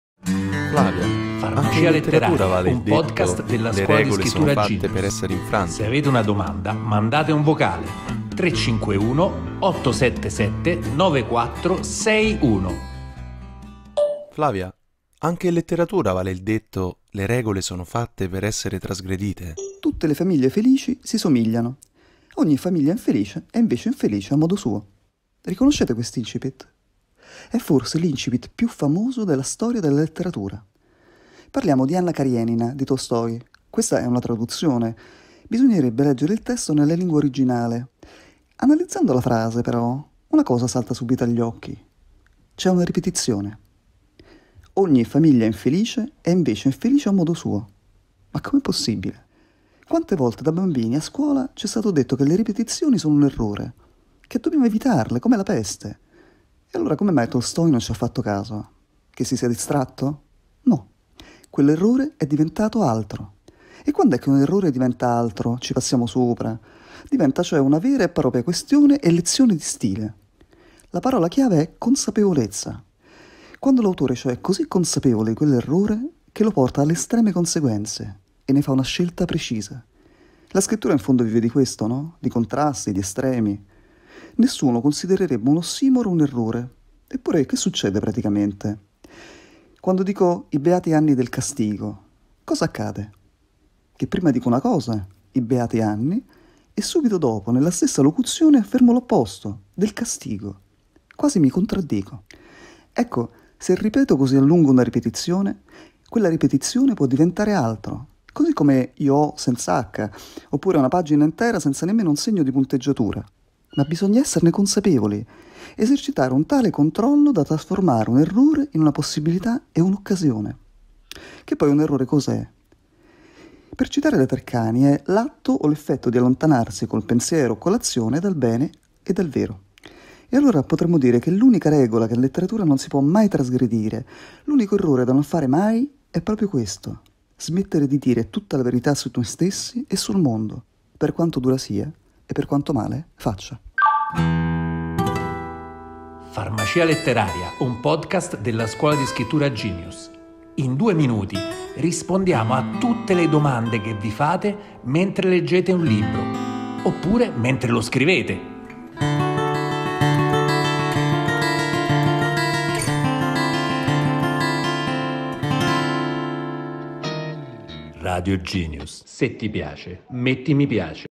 199 – TRASGREDIRE LE REGOLE [scrittura creativa]
Anche in letteratura vale il detto “le regole sono fatte per essere trasgredite”?
Risponde Flavia Ganzenua. Ha lavorato come dialoghista televisiva (Rai e Mediaset), ha scritto racconti per antologie collettive (Mondadori), riviste e blog (Nazione Indiana). Ha pubblicato la raccolta “La conta delle lentiggini” (CaratteriMobili editore). Conduce da anni laboratori di scrittura.